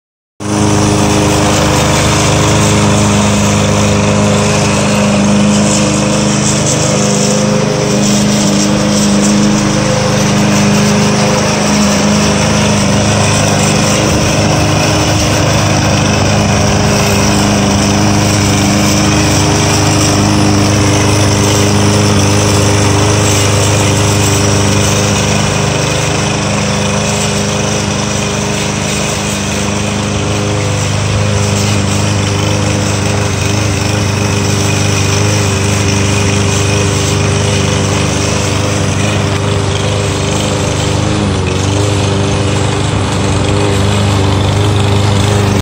Homemade ride behind lawn mower
Convert a walk behind lawn mower to ride behind